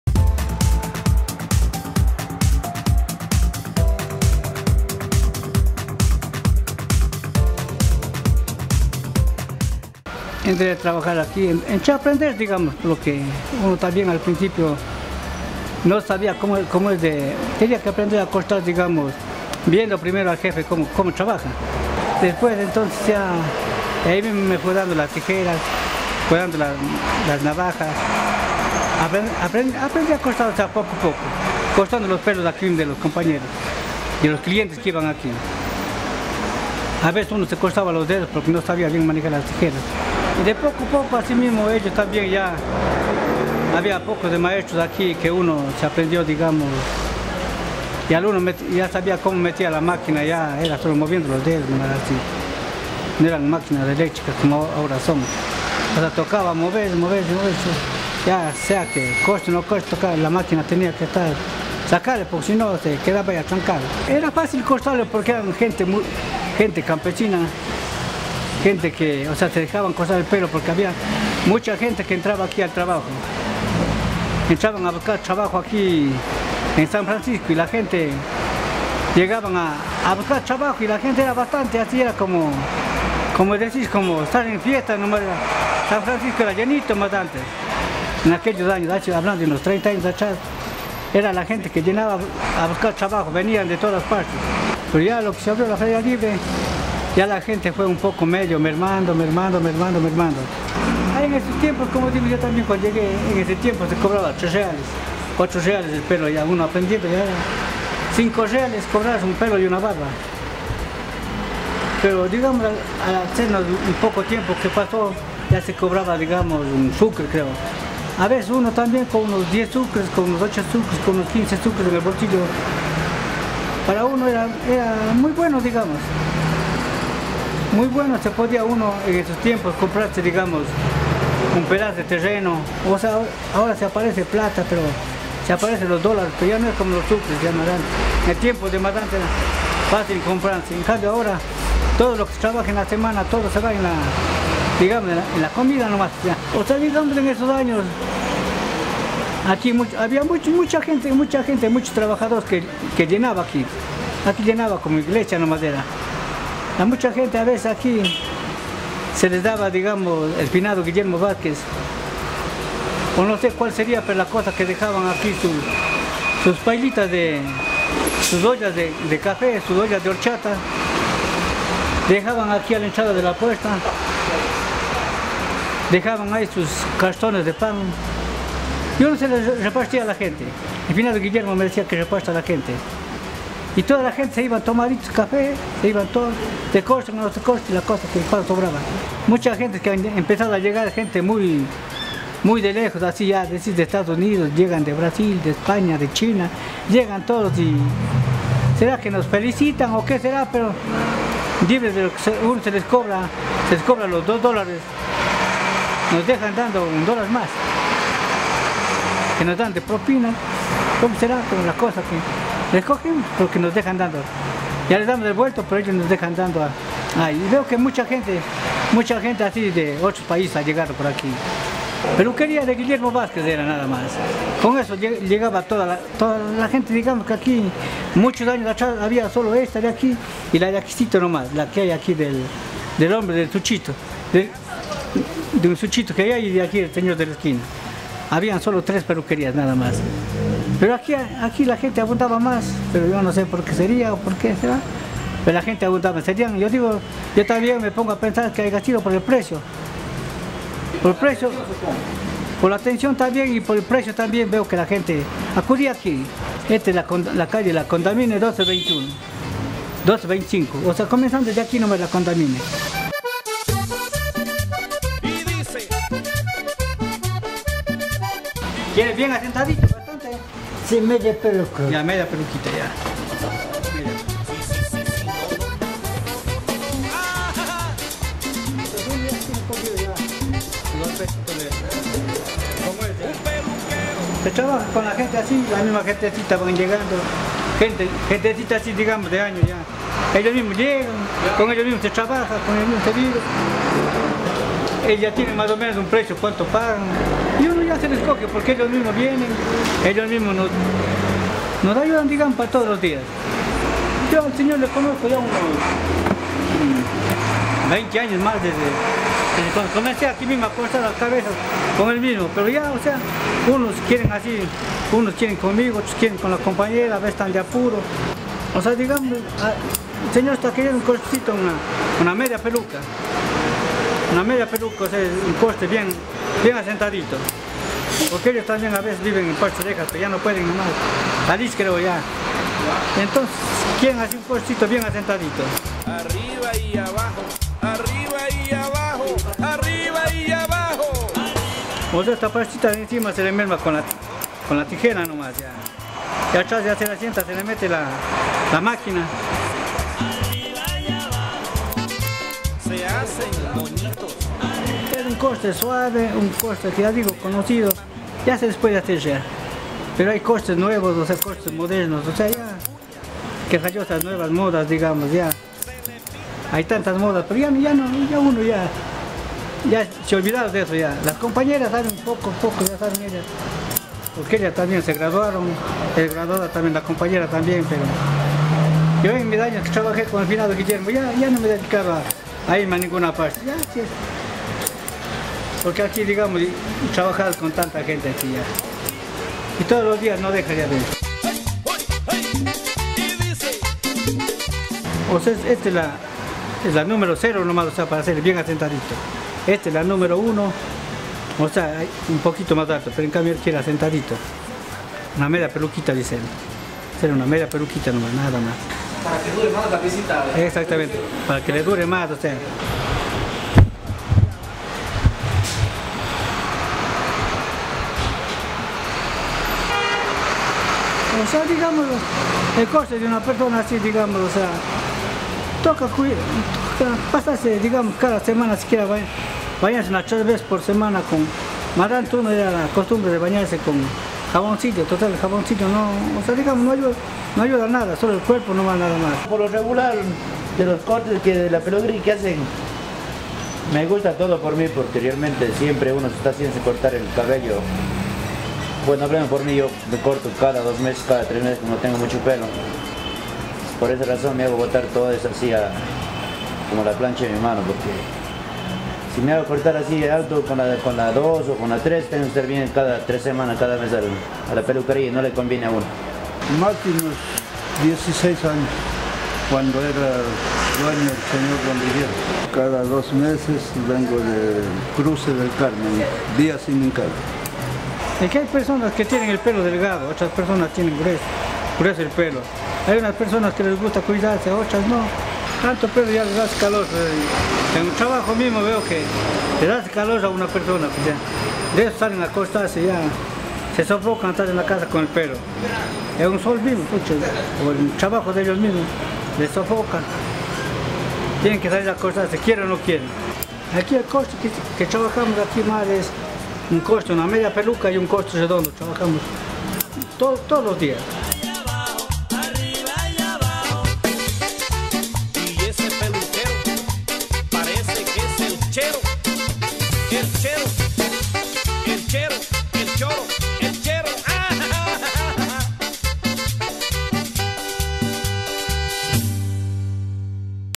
Bolívar Tobar Burgos (peluquería Los Arcos)
Bolívar Tobar Burgos un peluquero de los antiguos nos cuenta sus inicios y su historia en la peluquería Los Arcos.